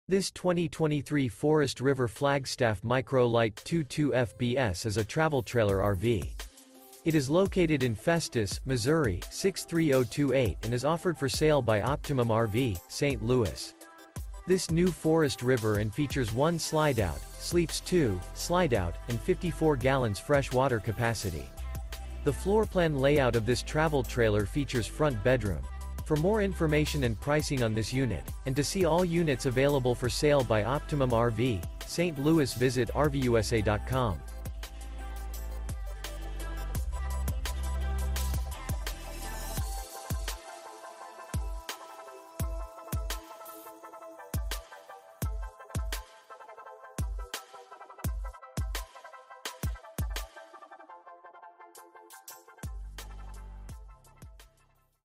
Marvelous 2023 Forest River Flagstaff Micro Lite Travel Trailer RV For Sale in Festus, MO
This 2023 Forest River Flagstaff Micro Lite 22FBS is a Travel Trailer RV. It is located in Festus, MO 63028 and is offered for sale by Optimum RV - St. Louis. This New Forest River and features 1 slideout, sleeps 2, Slideout, and 54 gal fresh water capacity. The floorplan layout of this Travel Trailer features Front Bedroom.